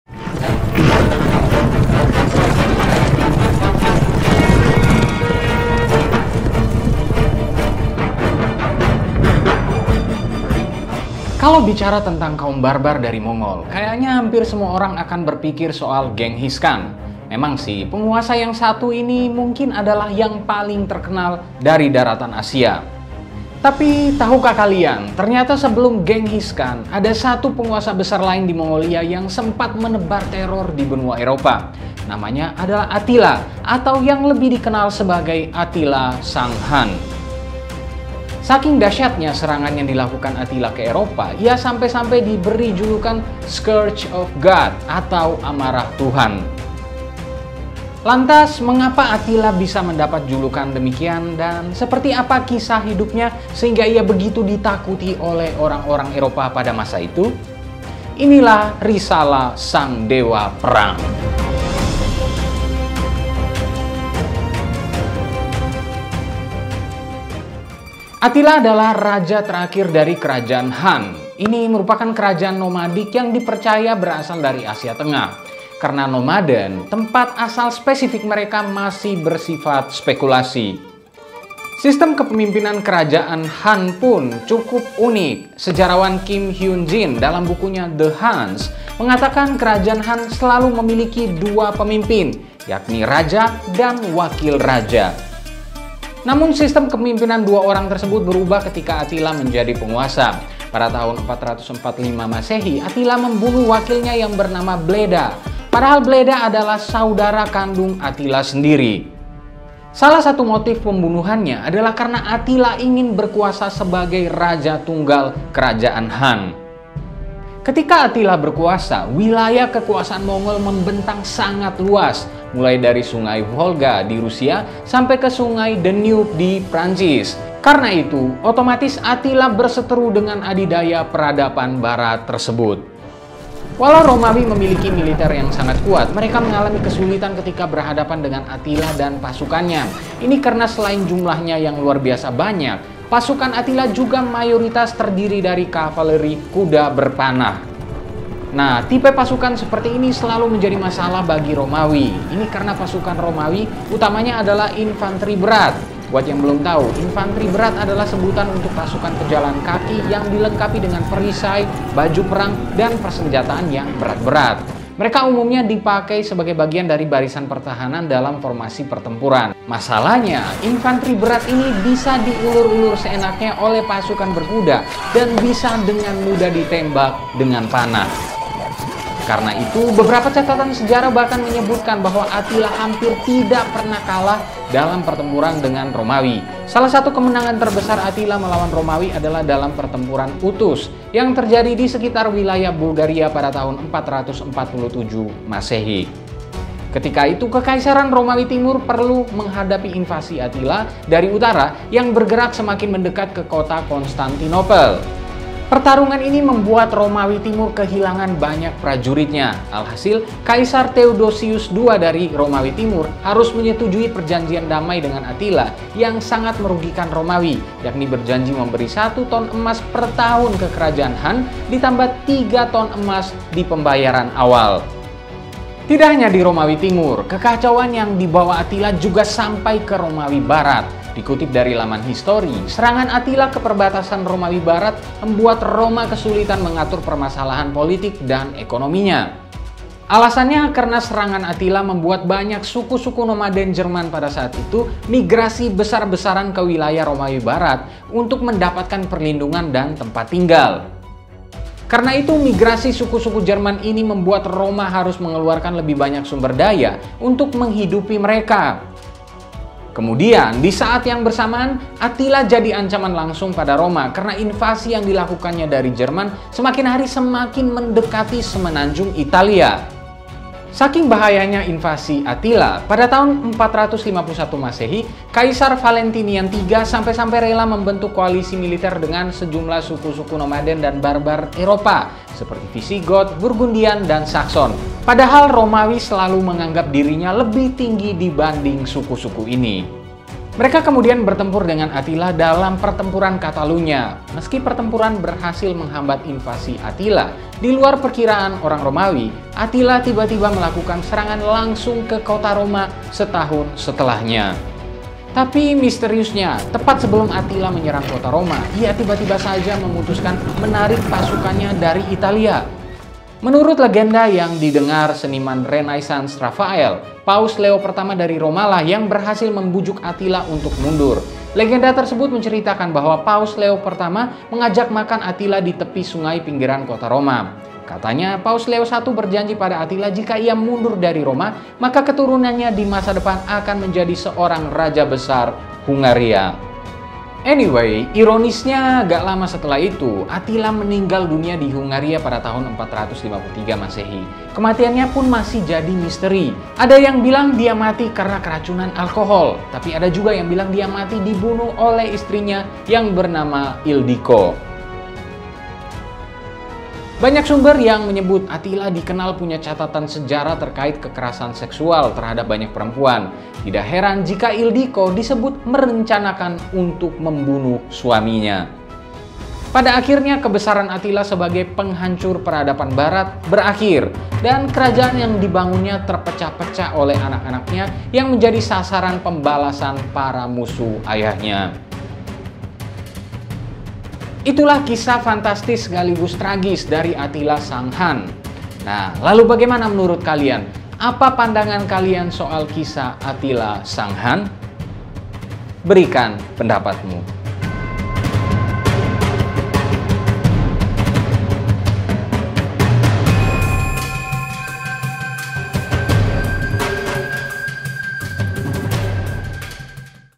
Attila Sang Hun: Dewa Perang yang Hampir Kuasai Dunia
Tahukah kalian? Ternyata sebelum Genghis Khan, ada satu penguasa besar lain di Mongolia yang sempat menebar teror di Benua Eropa. Namanya adalah Attila, atau yang lebih dikenal sebagai Attila sang Hun.

Saking dahsyatnya serangan yang dilakukan Attila ke Eropa, ia sampai-sampai diberi julukan “Scourge of God”, atau Amarah Tuhan.

Lantas, mengapa Attila bisa mendapat julukan demikian dan seperti apa kisah hidupnya sehingga ia begitu ditakuti oleh orang-orang Eropa pada masa itu? Inilah risalah Sang Dewa Perang!